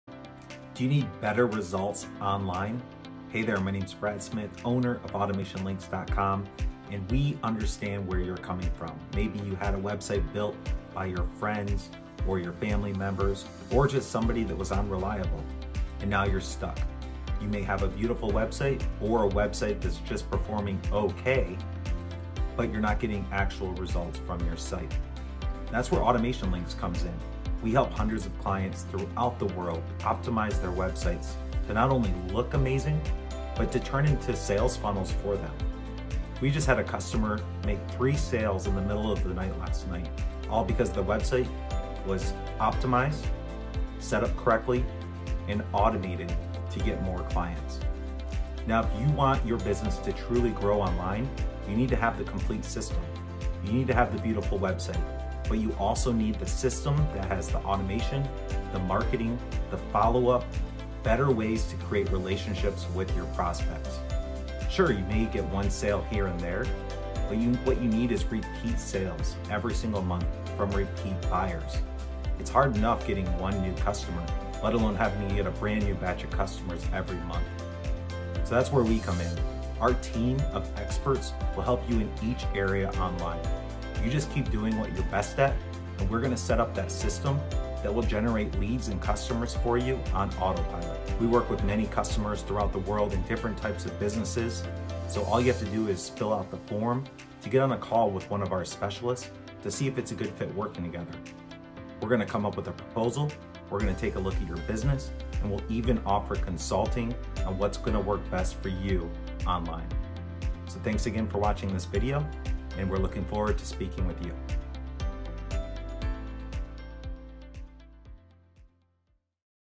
How to build a website for business owners
In this video, Brad Smith shows you why it's important to have a website online.

What you'll learn if you watch this video:
1. Why a website can get you more customers on auto-pilot
2. How to build better relationships with your website visitors
3. How to use automation on your website to get 10x more customers